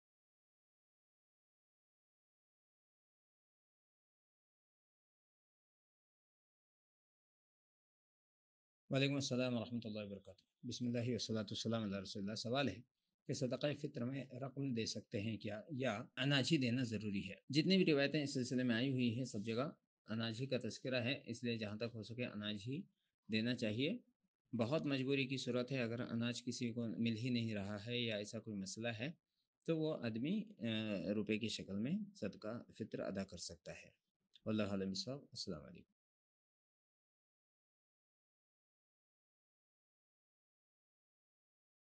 Sadqa-E-Fitr Me Raqm De Sakte Hai Ya Anaaj hi Dena Zaroori Hai?
Jam'iyyat Ahle Hadees, Aurangabad
Presents

Answer By:
Fazilatushshaikh Kaleem Mohammadi Hafizahullah

📩 _What's App Broadcast:_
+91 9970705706